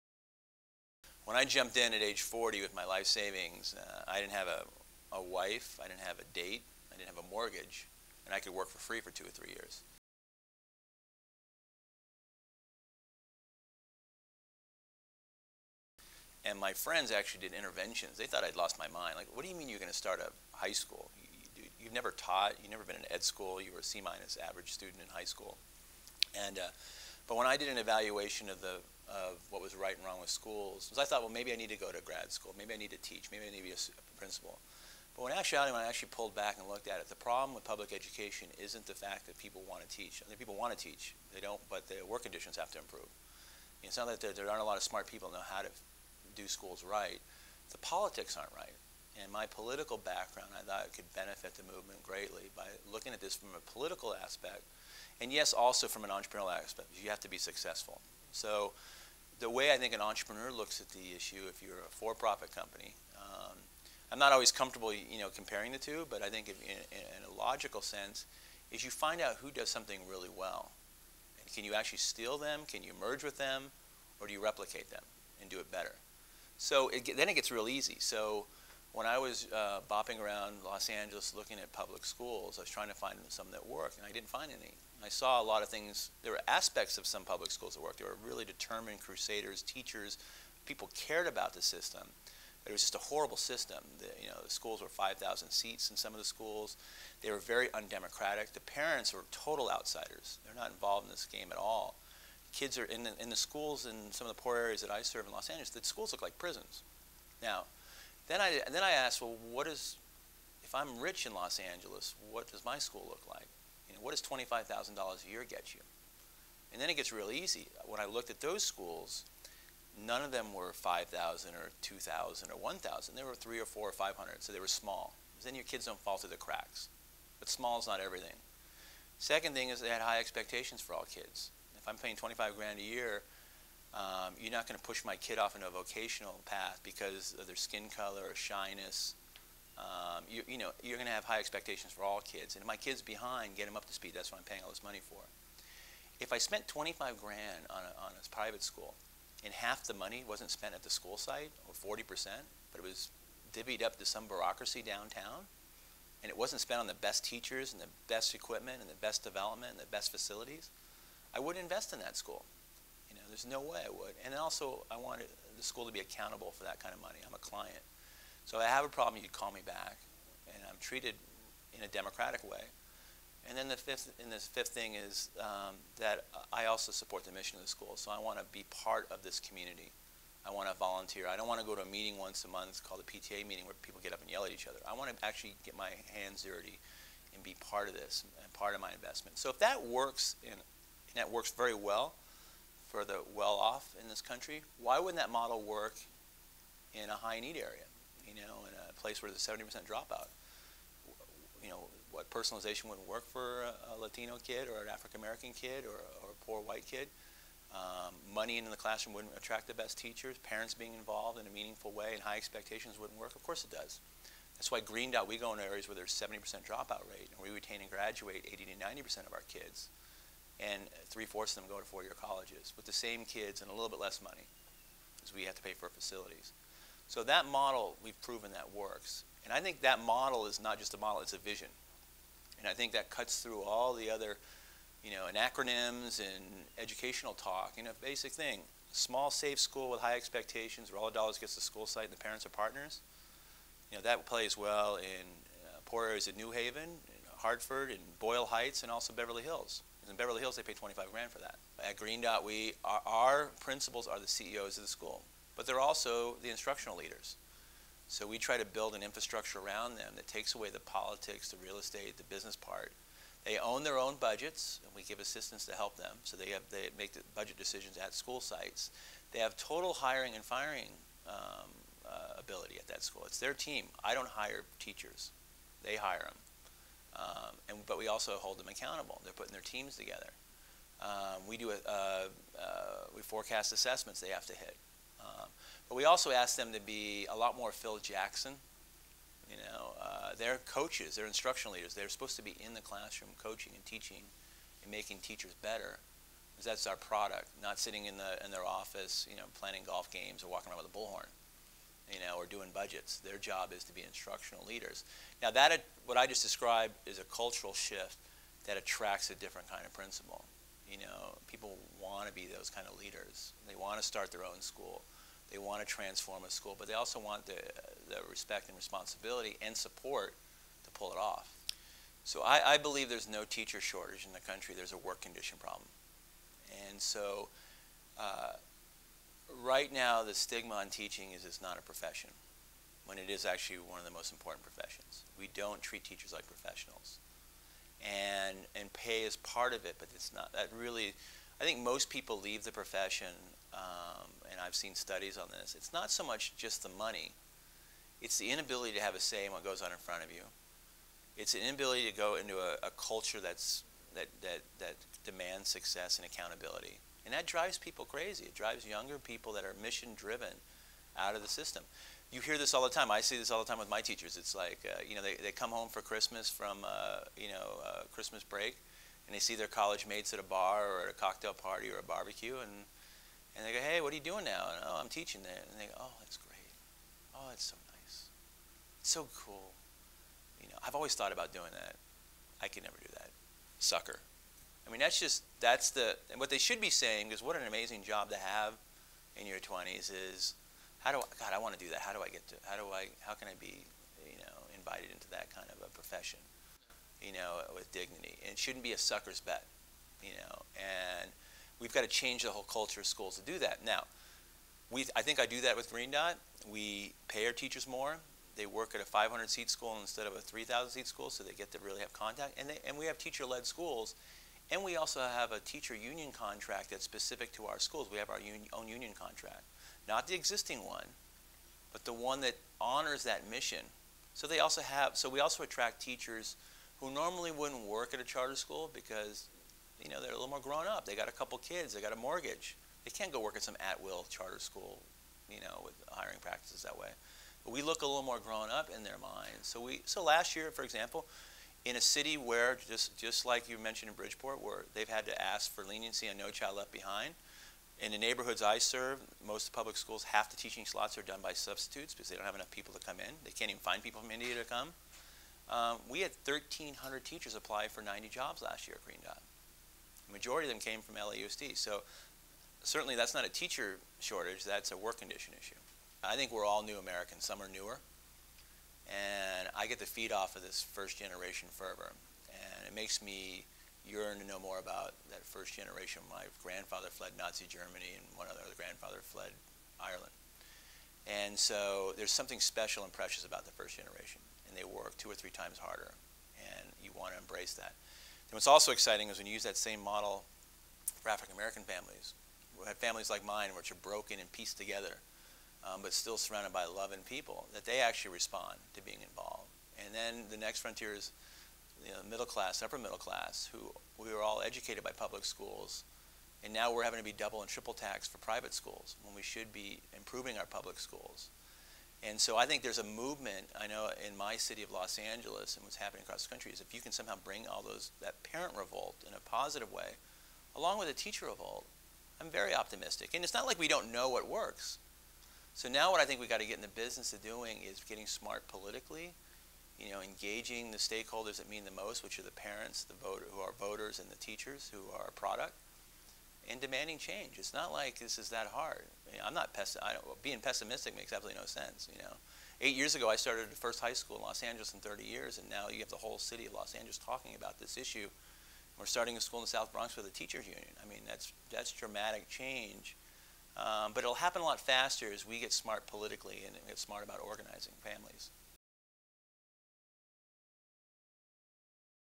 ConnCAN "First Person" Interview: Steve Barr
ConnCAN.org - ConnCAN's one on one interview with Steve Barr, Founder and CEO of Green Dot Public Schools.